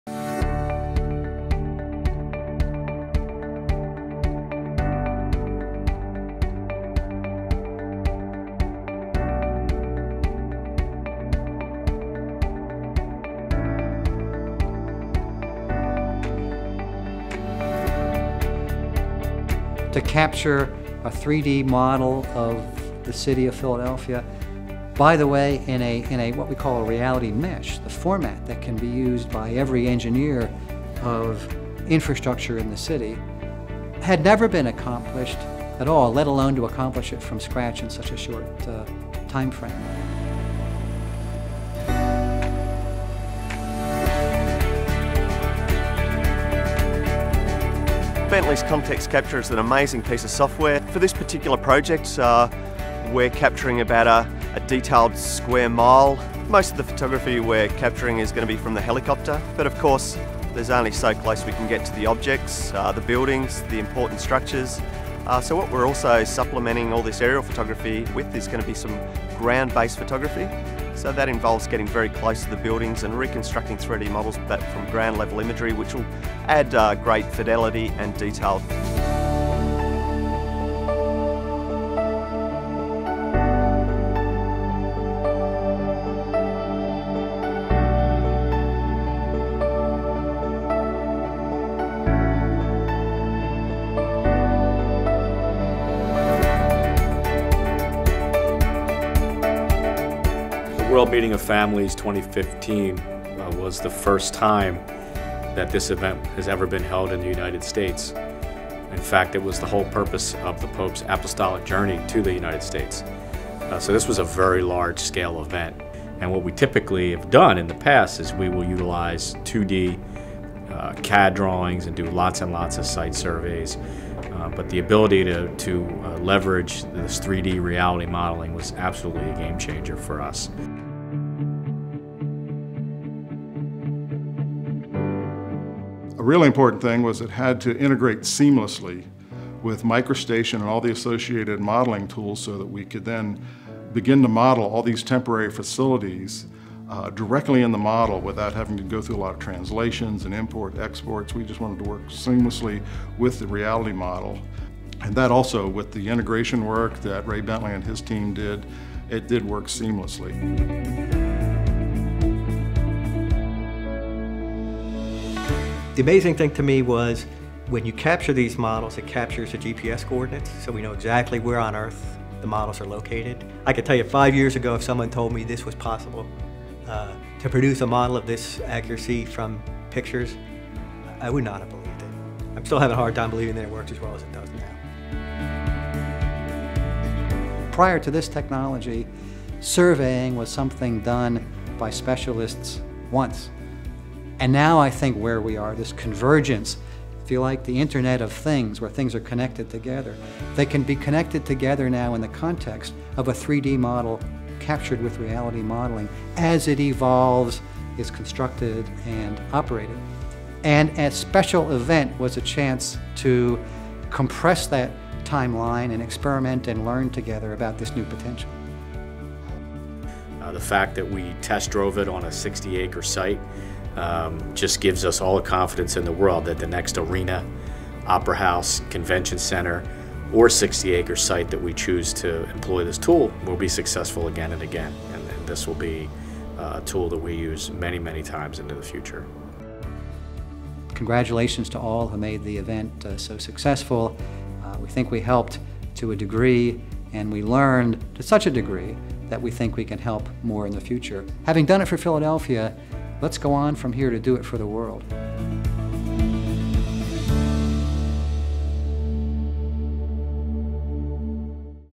Reality Modeling for the Philadelphia Papal Visit
ESM Productions credits Bentley Systems' reality modeling technology in its successful engineering of the Philadelphia Papal Visit.